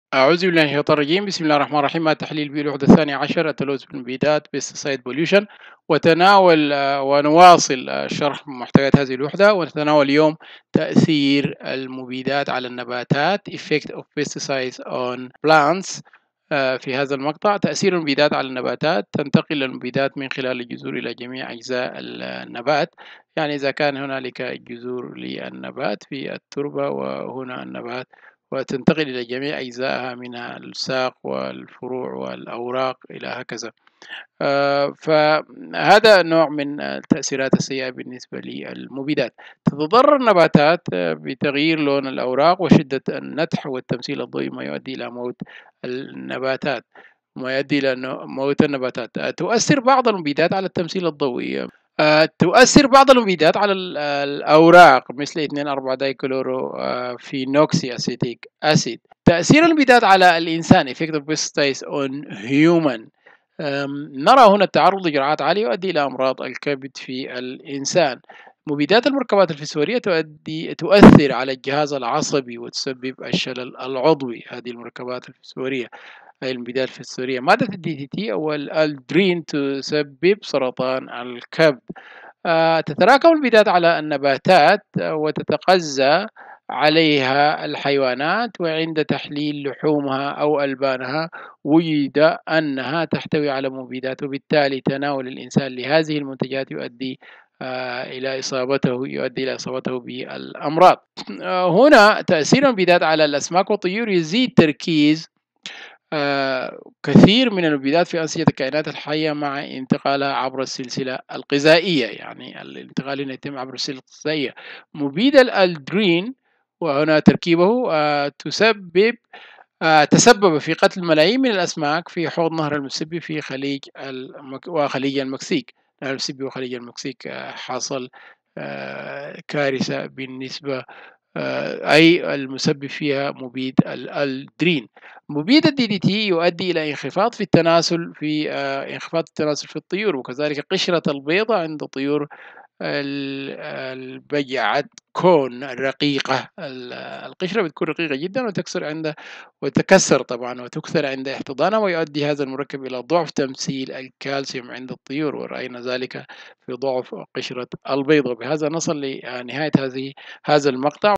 التحليل البيئي 2 | الوحدة 12 | تأثير المبيدات على البيئة الحية وغيرها - 1
الوحدة الثانية عشرة: التلوث بالمبيدات
تأثير المبيدات على البيئة الحية
1. تأثير المبيدات على النباتات
2. تأثير المبيدات على  الانسان
3. تأثير المبيدات على الأسماك والطيور